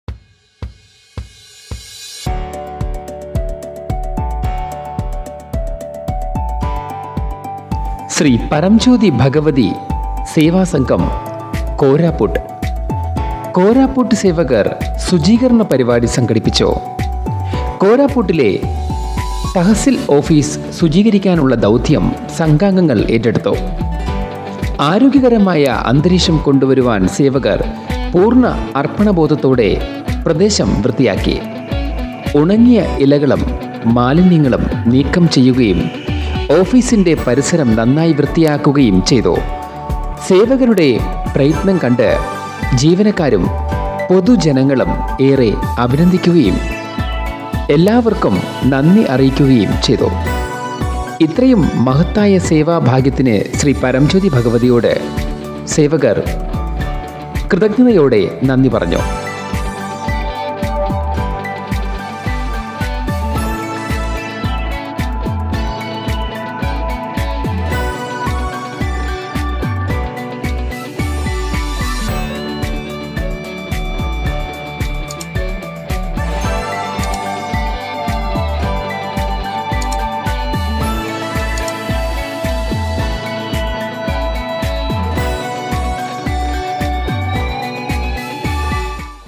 Malayalam: Koraput Seva Sangha| Organised Cleanliness drive program @ Tahasil Office with dedication
Sri Paramjyoti Seva Sangha members across length and breath of India helping the needy in many ways. These members are those people who are seekers of enlightenment spreading a wave of Oneness among fellow Indians.